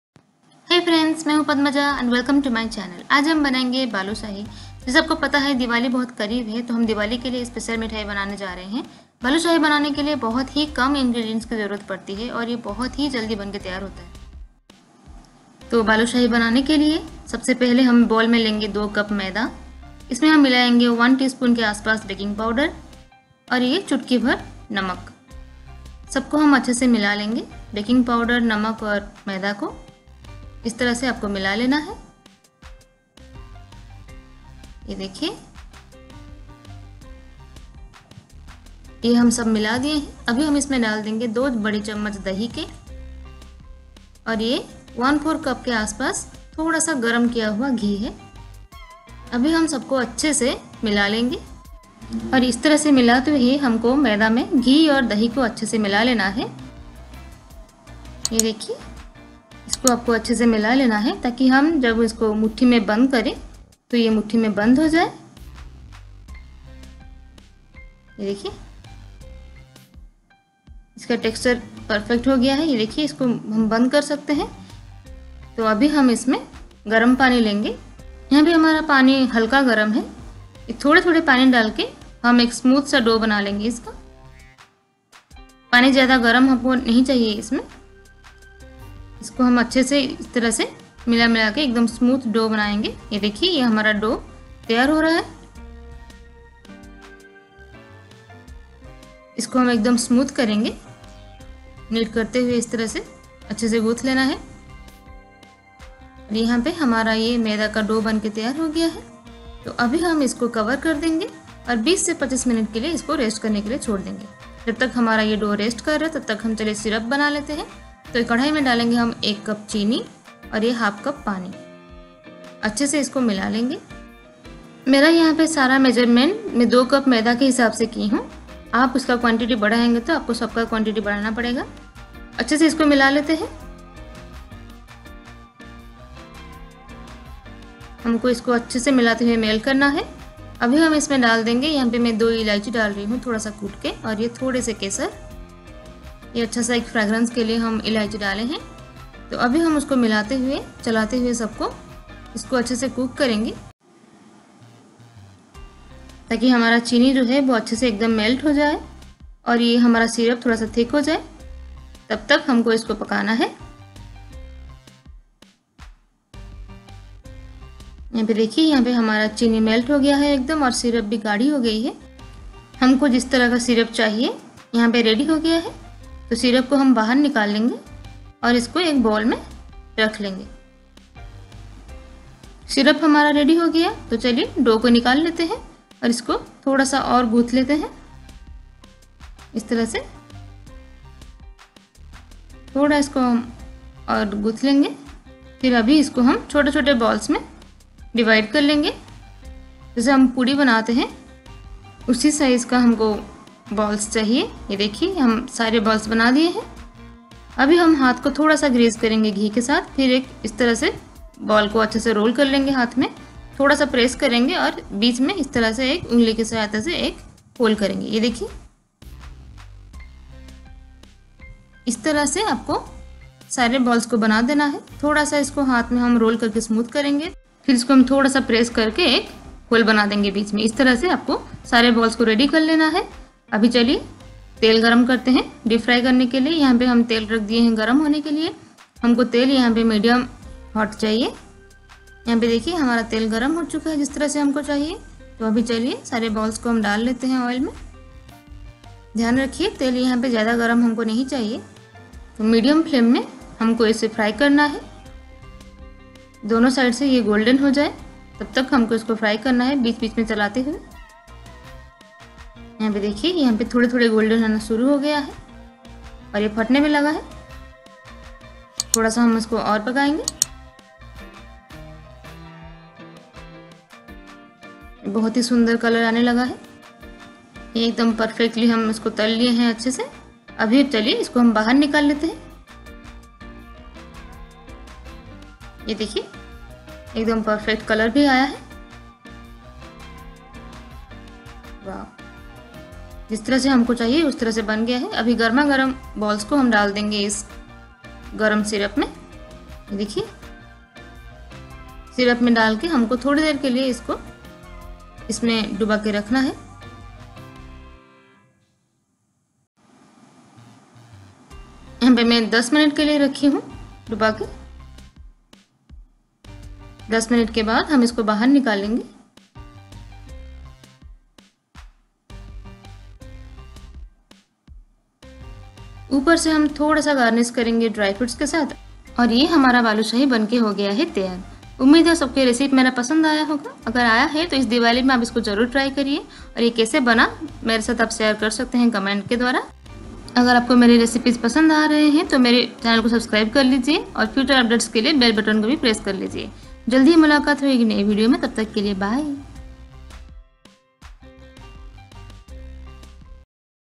Balushahi recipe for Diwali|इस दिवाली में घर पे बनाये हलवाई जैसी बालूशाही आसानी से ||Padmaja kitchen
Ingedients:
1.Maida/All purpose flour-2 cups
2.Baking powder-1tsp
3.Salt 1/2 tsp
4.Curd/yogurt-2tbsp
5.Ghee 1/4 cup melted
6.Warm water as required

Method:
1.In a mixing bowl add 2cups of maida/all purpose flour,baking powder-1tsp,salt 1/2 tsp.Mix everything well.
2.Then add 2tbsp curd,melted ghee 1/4 cup .Mix everything well till getting holding texture.
3.Now add warm water little by little & knead a smooth dough. Cover & allow it rest for 20 minutes.
4.Now heat a pan . Add 1 cup sugar,1/2 cup water,cardamom-2, saffron-10 strands.give it a mix & cook it till sugar melted & till we get one string consistency.
5.Now sugar syrup is ready keep aside.
6.Now take the dough & knead a little & divide it into small balls like puris.
7.Now grease your palms with ghee.Take a ball roll it smooth & make dent in centre.
8.Like this make all the balls like this.
9.Now heat a pan & add oil for deep frying & heat it to medium.
10.Then add the ball to the oil & fry till golden in medium flame.
11.Now take out & put it on warm sugar syrup.
12.keep it like this for 10 minutes.
13.Now take out Garnish with chopped nuts & enjoy...